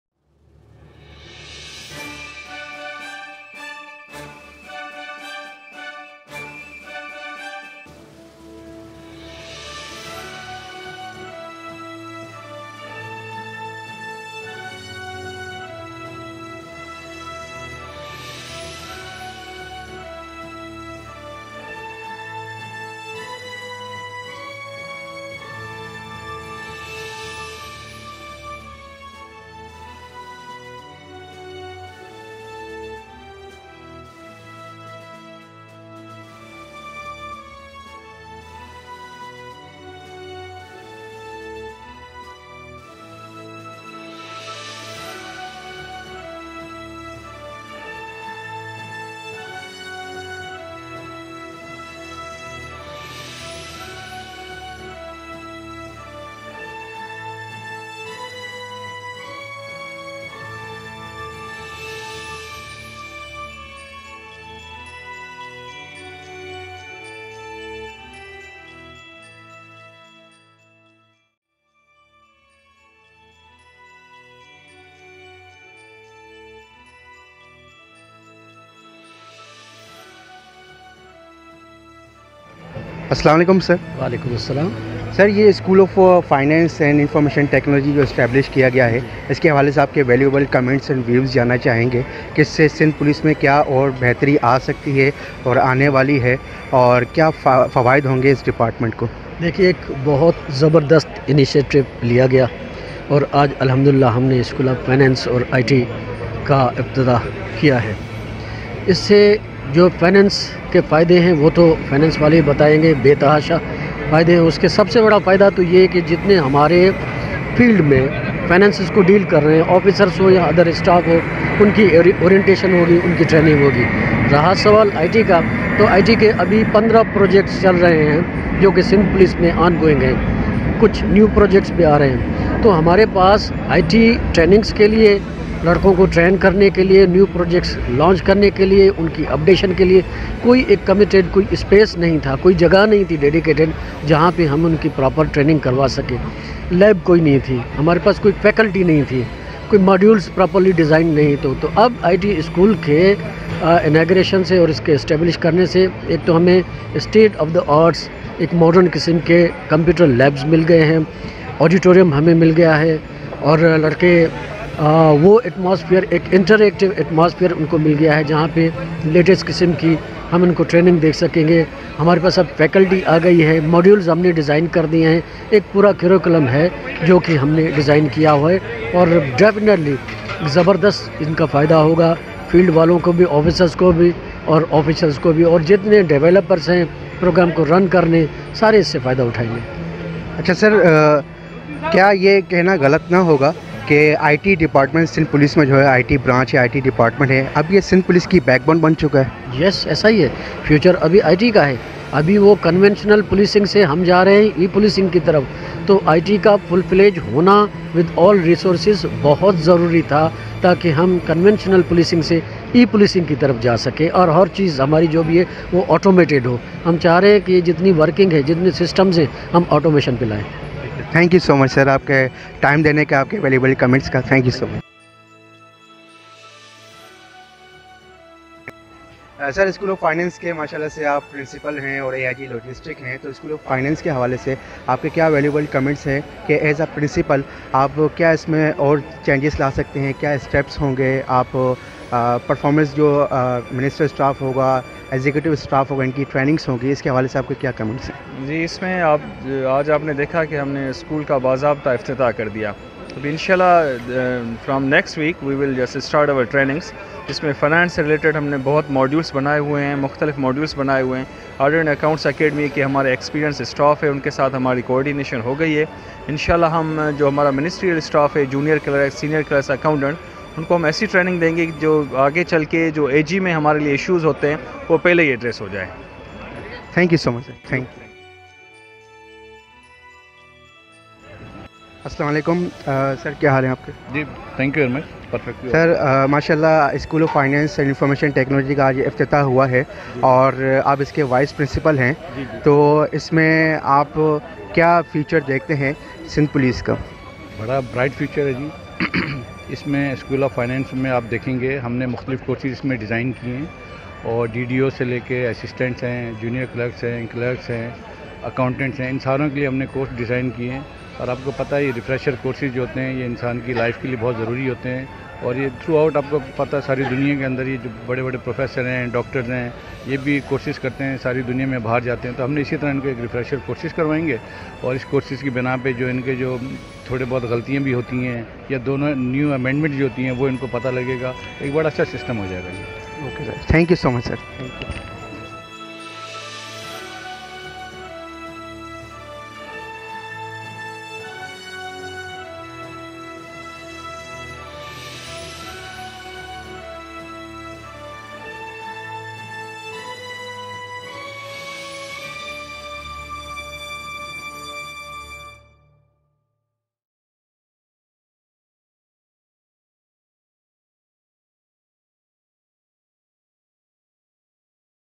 Interviews of Police Officers | School of Finance and IT CPO Karachi | Sindh Police
Interviews Regarding School of Finance and IT CPO Karachi | Sindh Police
Sindh Police has established School of Finance and Information Technology in CPO, Karachi.
This is a valuable video for anyone interested in the police force, as it will provide you with an inside look at some of the challenges and achievements of the police force. If you're looking for information about the School of Finance and IT CPO Karachi, or want to learn more about the police force in general, then be sure to watch this video!
Police Officers:
DIGP I.T CPO, Sindh Mr. Fida Hussain Mastoi , PSP
AIGP Logistics, CPO/Principal School of Finance and I.T CPO Dr. Samiullah Soomro, PSP
DSP/Vice Principal School of Finance and I.T CPO, Karachi Mr. Tariq Mughal, SPS.
Host: Rizwan Ali Khan